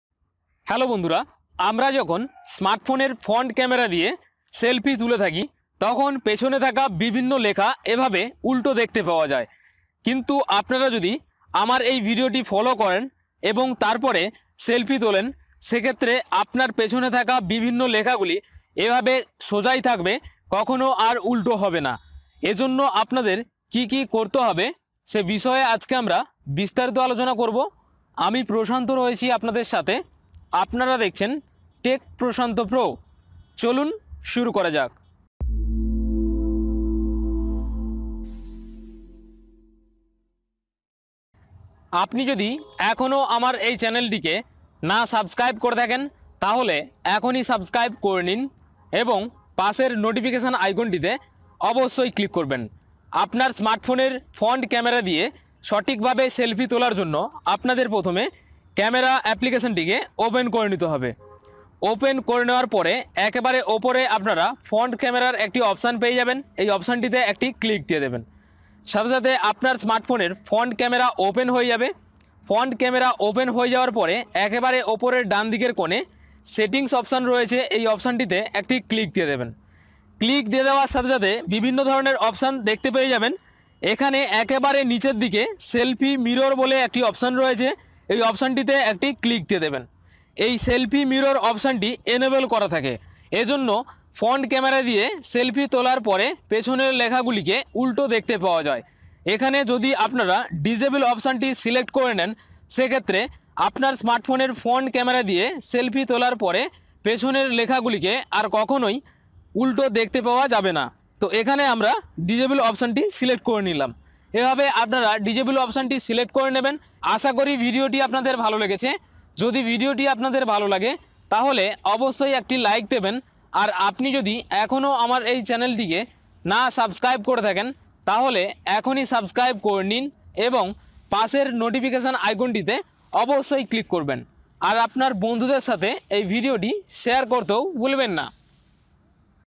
Font Camera Selfie Mirror Problem Solve | Camera Selfie Settings (Bangla)
Hello friends, we all use smartphone of different reputed companies. When take a selfie with the font camera of this smartphone, the text on the back of the selfie can be seen mirror. If you make some settings in android, the text on the back of the selfie will never look the mirror.